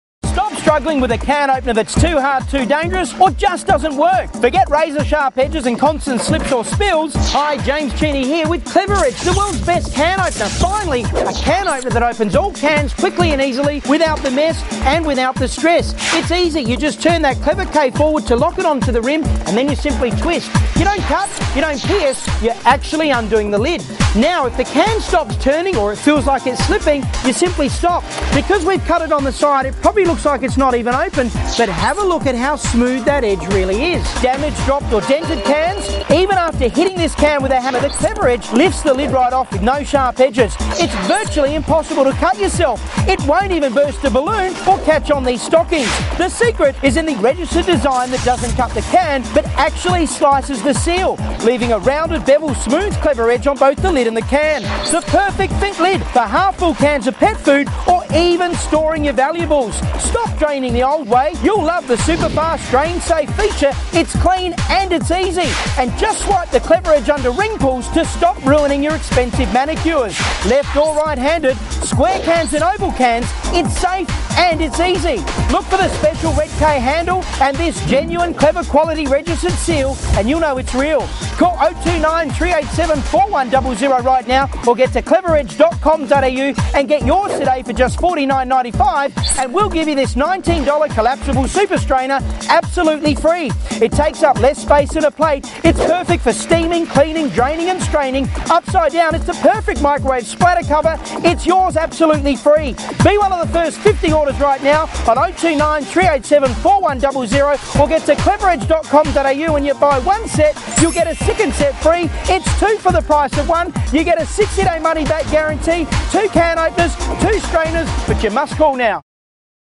Kleva Edge
Now, with Kleva Edge Can opener, you can quickly open cans of any shape and size in seconds. Even those moderately dented ones!